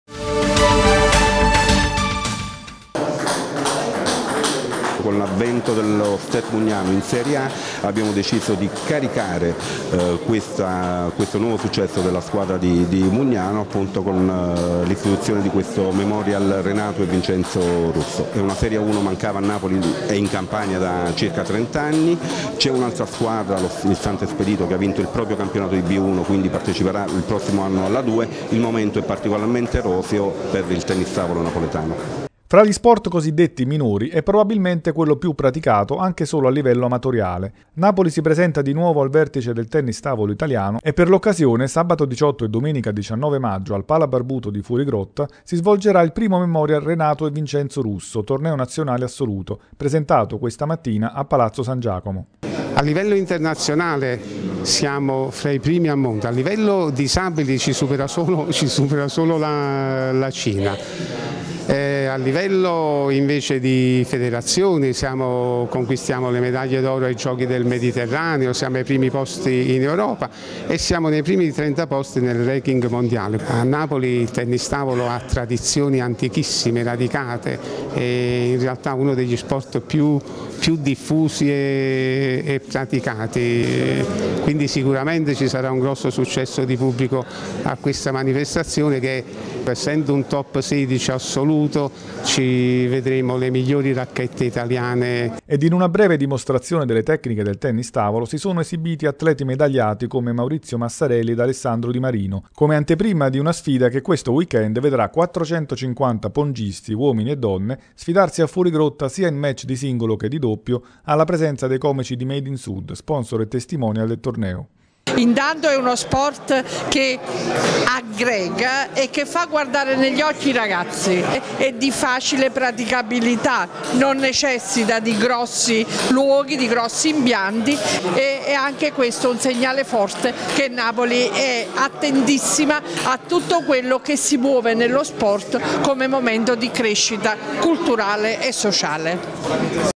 Il tennis tavolo di nuovo protagonista a Napoli
Napoli si presenta di nuovo al vertice del tennis tavolo italiano, e per l'occasione, sabato 18 e domenica 19 al PalaBarbuto di Fuorigrotta si svolgerà il 1° memorial "Renato e Vincenzo Russo -- torneo nazionale assoluto".